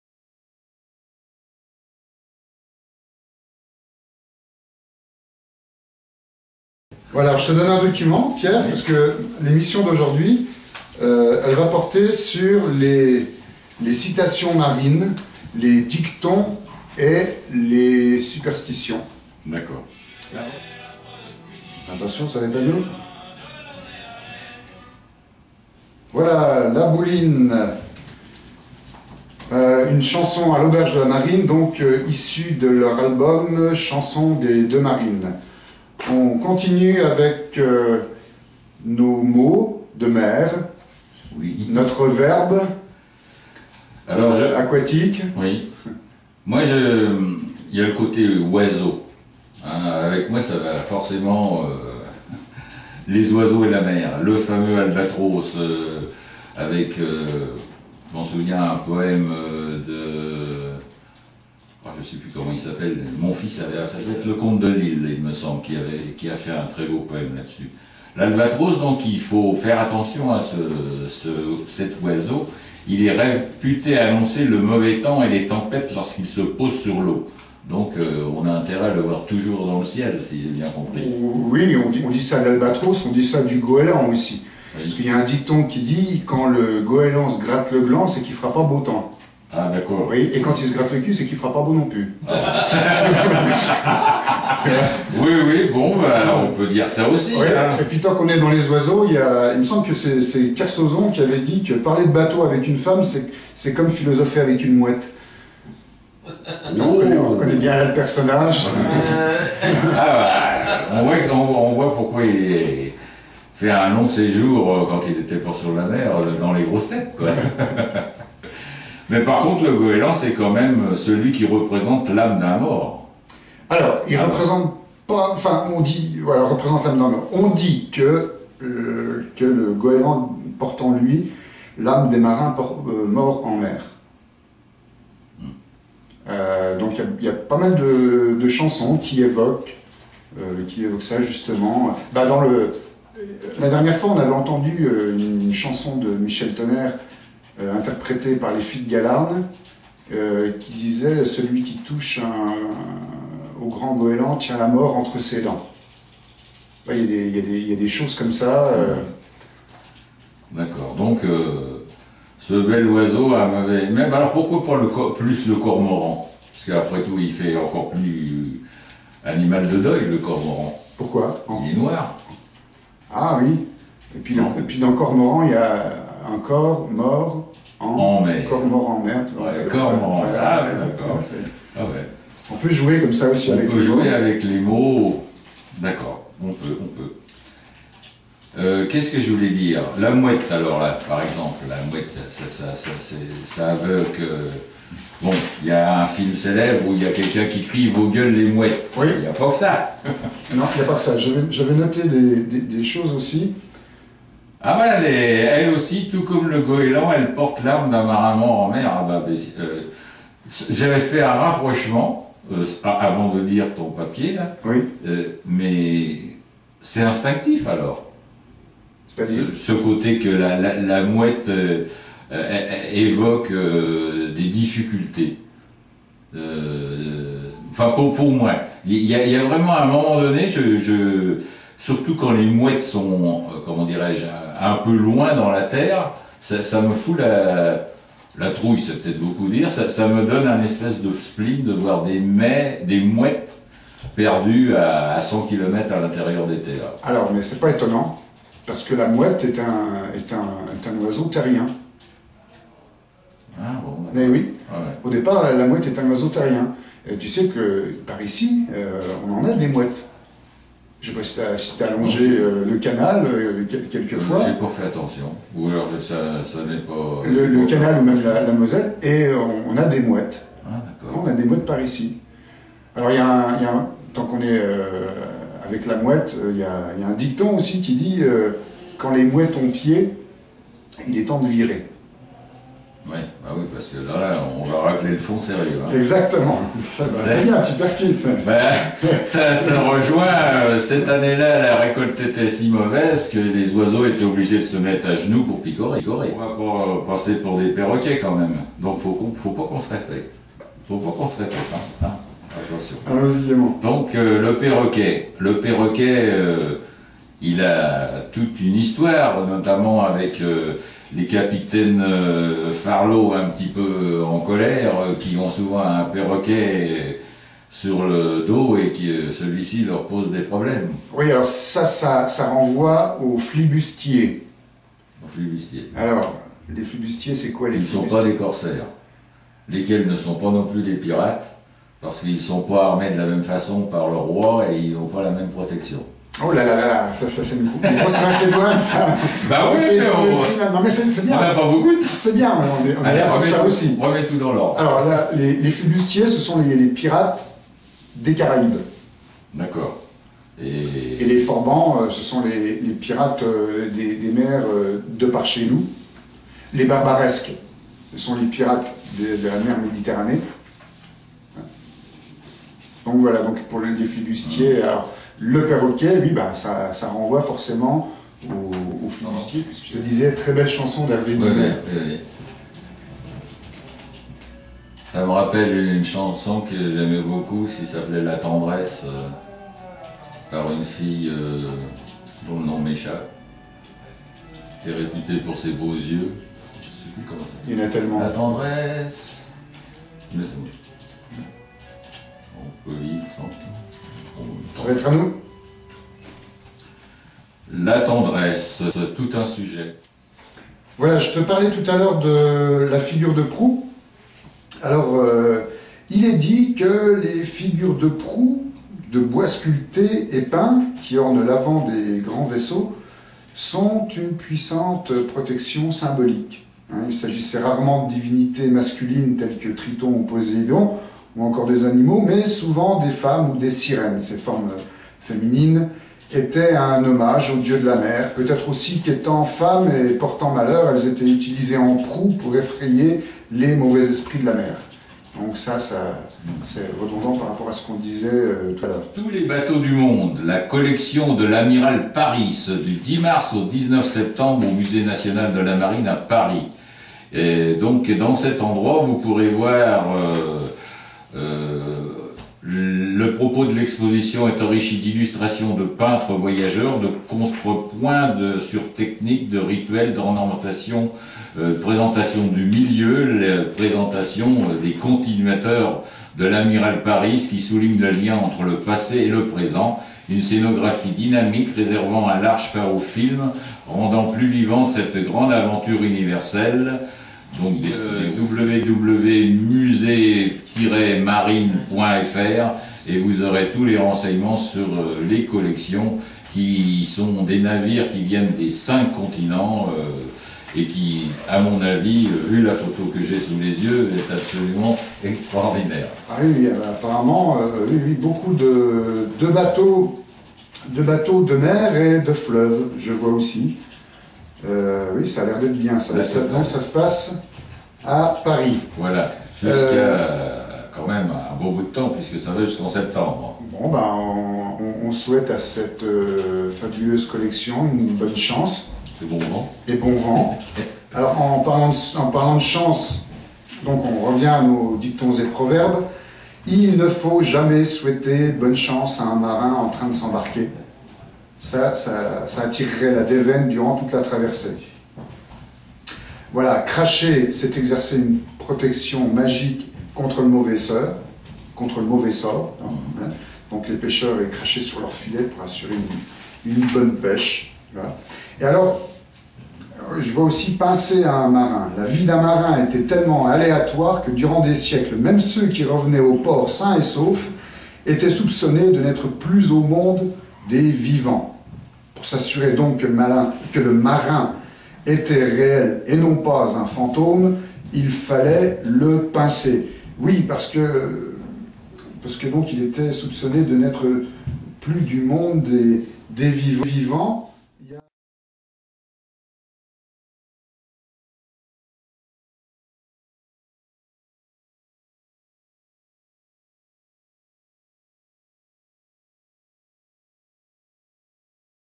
FAJET 27 03 10 FIN
Rohan invité de radio Fajet Nancy

Spécial chants de marins
"Proverbes, dictons et superstitions"